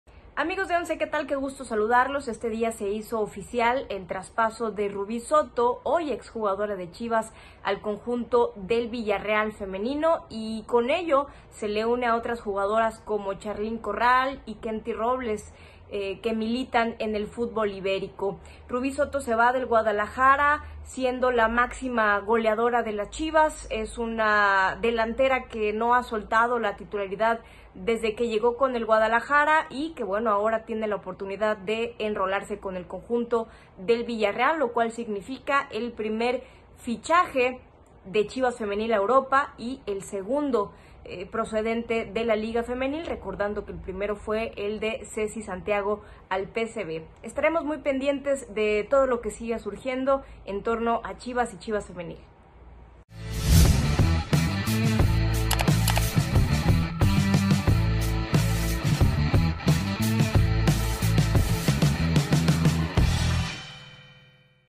Chivas Femenil | Rubí Soto, de Chivas Femenil, a Europa | ONCEDiario
Se hizo oficial el transpaso de Rubi Soto, máxima goleadora de Chivas Femenil, al Villarreal Femenino.